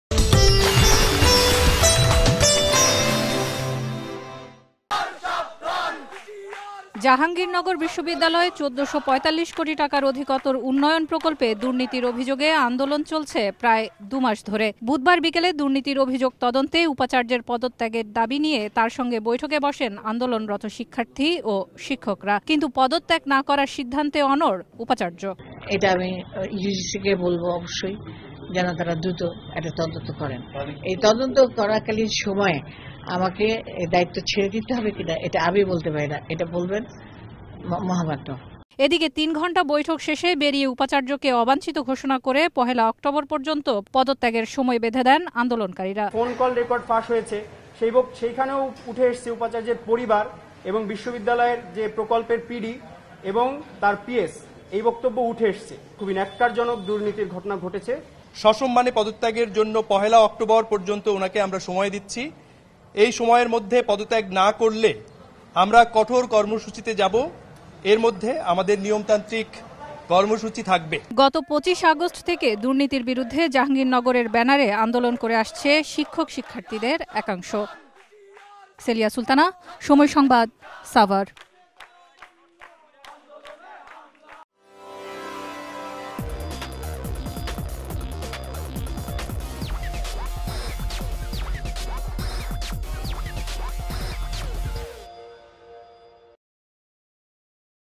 আন্দোলনের মুখে পদত্যাগ করবেন না জাবি উপাচার্য | Jahangirnagar University | Somoy TV
আন্দোলনের মুখে পদত্যাগ করবেন না বলে সাফ জানিয়ে দিয়েছেন জাহাঙ্গীরনগর বিশ্ববিদ্যালয়ের উপাচার্য। দুর্নীতির অভিযোগে আন্দোলনকারী শিক্ষার্থী ও শিক্ষকদের সঙ্গে বৈঠক শেষে সাংবাদিকদের এ কথা জানান তিনি। এদিকে তাকে অবাঞ্চিত ঘোষণা করে পদত্যাগ করতে ১লা অক্টোবর পর্যন্ত পদত্যাগের সময় বেধে দিয়েছেন আন্দোলনকারীরা।